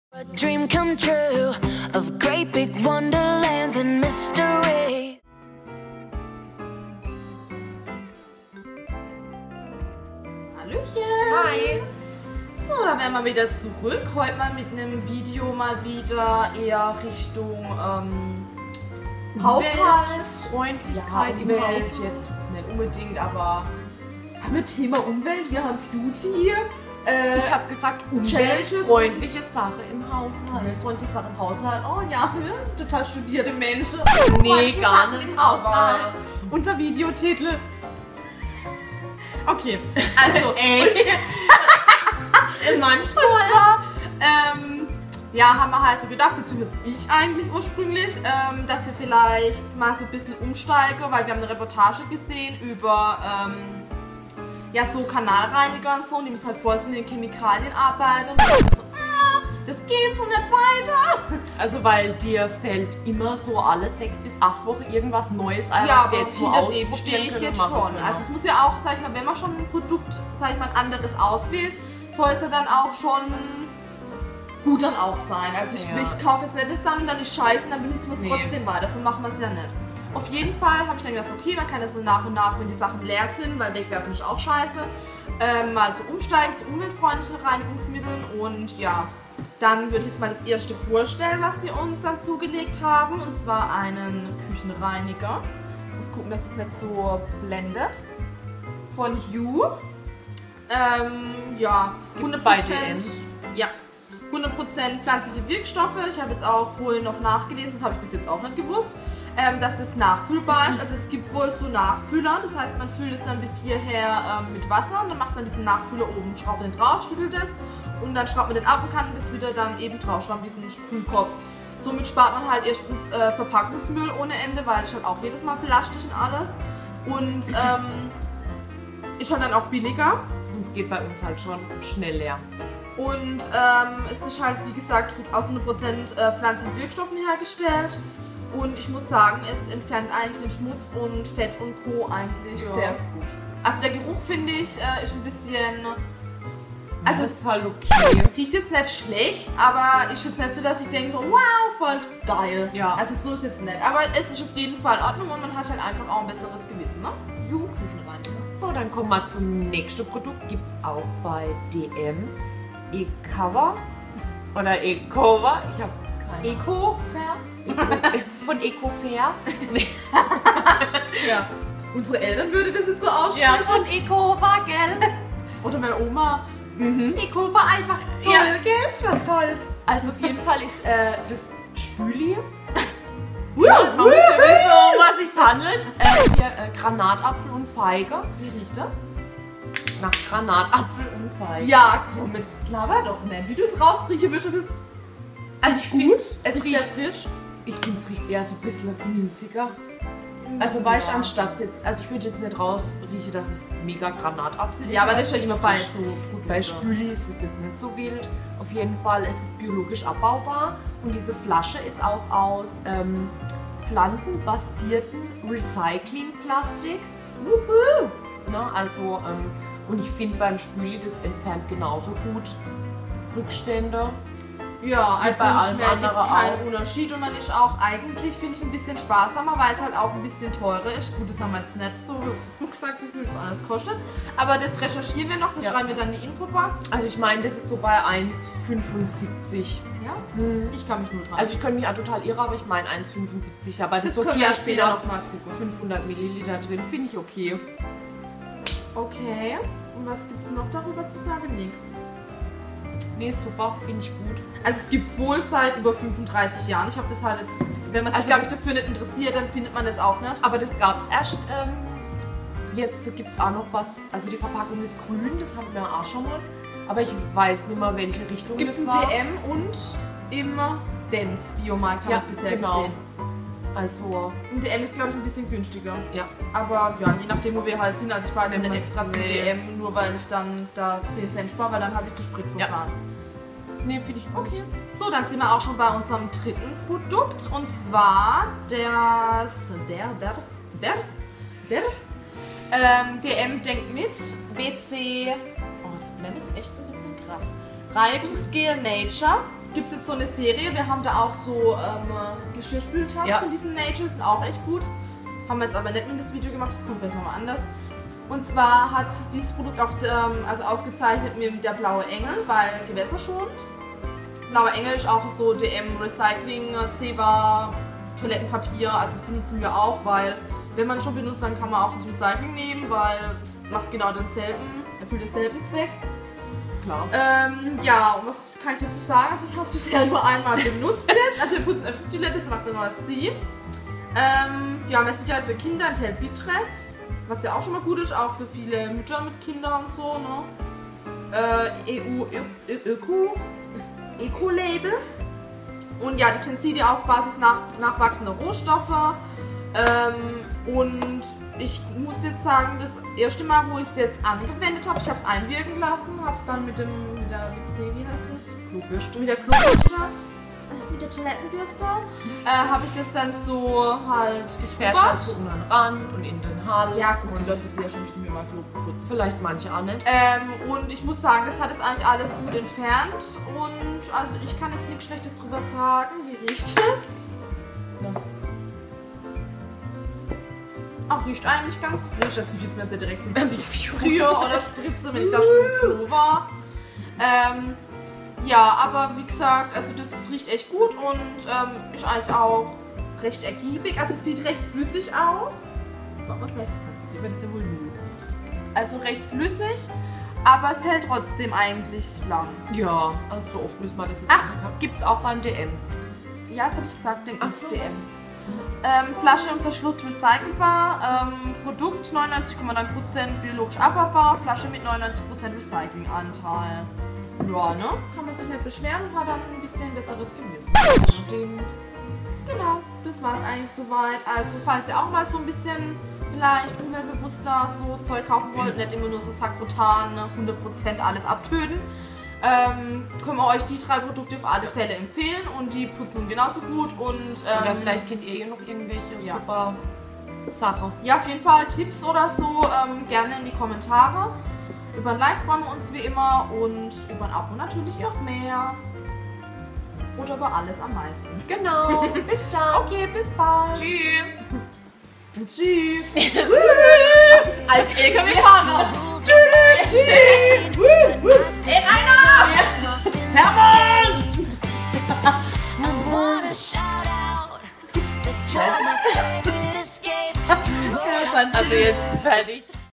TOP 3 umweltfreundliche Reinigungsmittel | nachhaltig, öko
Hallo liebe Freunde und baldige Freunde,

wir haben endlich wieder ein Video gedreht, dieses mal mit etwas informativen Inhalten (muss auch mal sein!). :)

Falls ihr noch Tipps zu anderen umweltfreundlichen Produkten habt: IMMER HER DAMIT!

Bis ganz bald
Lu & Chanti :-*